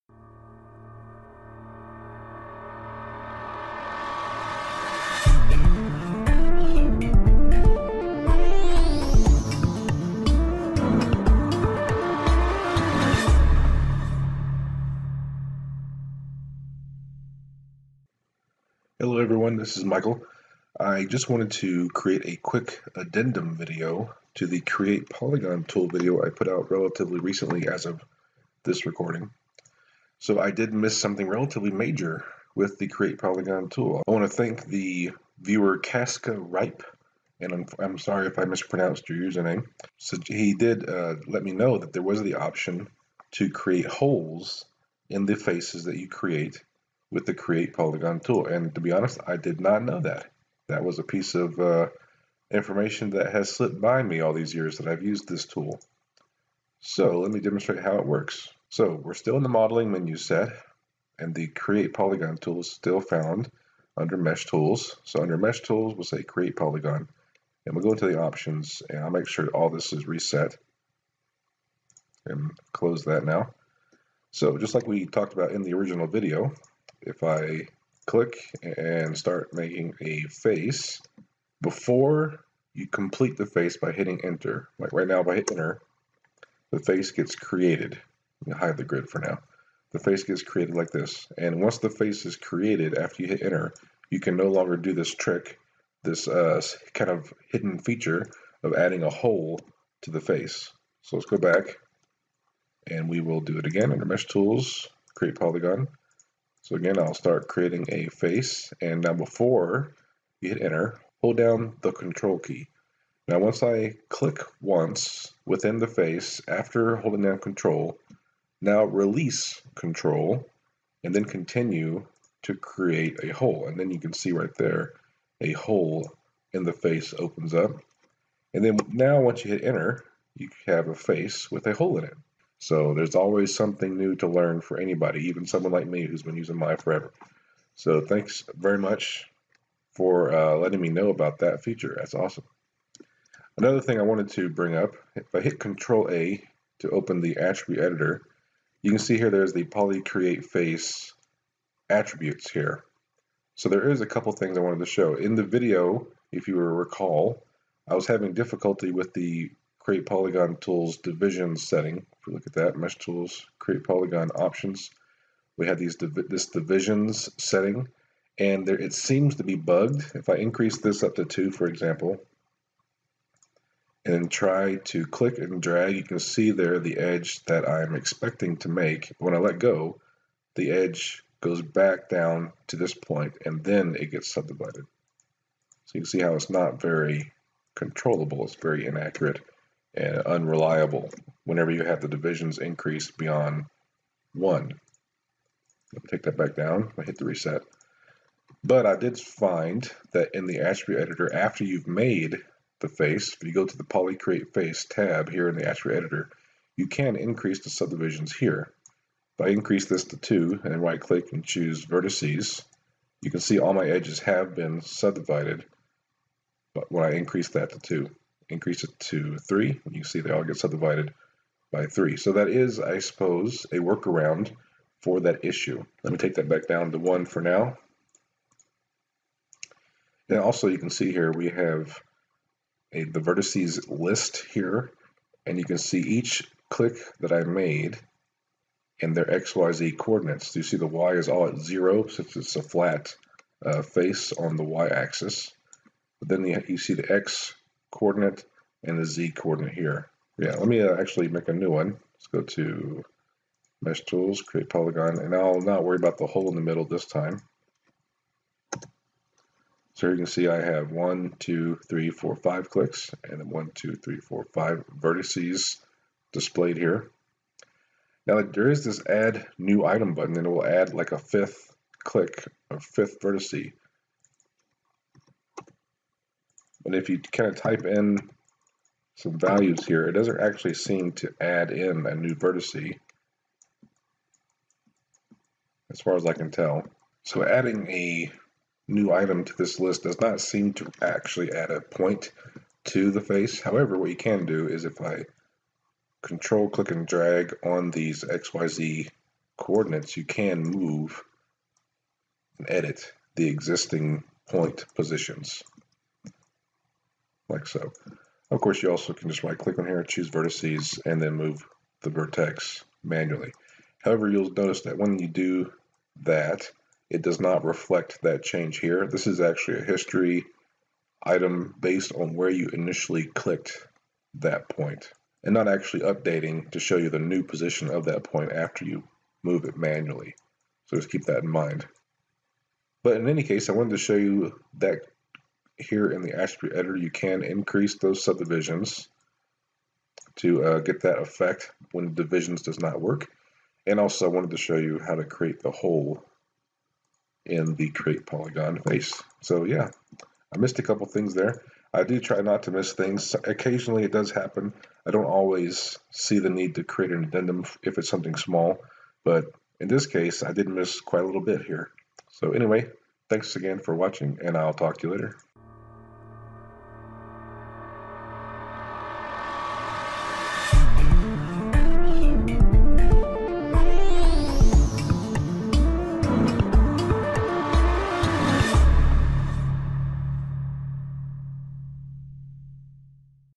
The Maya Toolbelt - Create Polygon Tool Addendum
Oops! I missed something when I made the Create Polygon Tool video! Here's an update showing a couple of features of the tool that I missed the first time through.